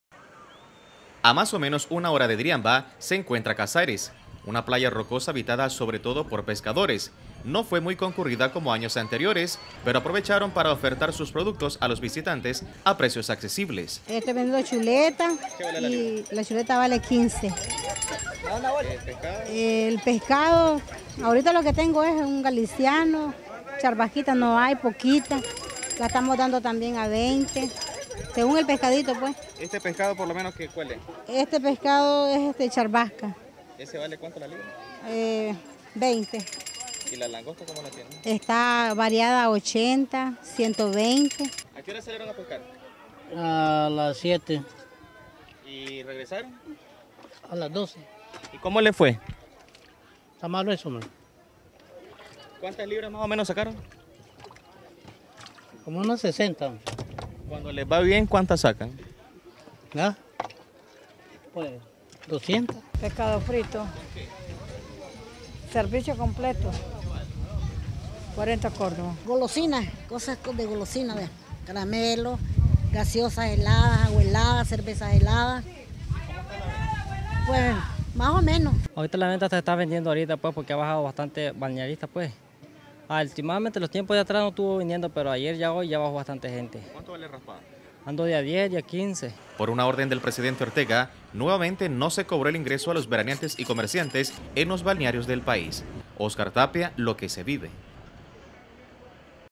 CASARES, BUENAS VENTAS Y BAJOS PRECIOS EN SEMANA SANTA
ACA ES UNA ZONA DE PESCADORES POR LO TANTO LOS MARISCOS SE ENCUENTRAN A BAJOS PRECIOS Y LOS PESCADOS FRITOS TAMBIEN.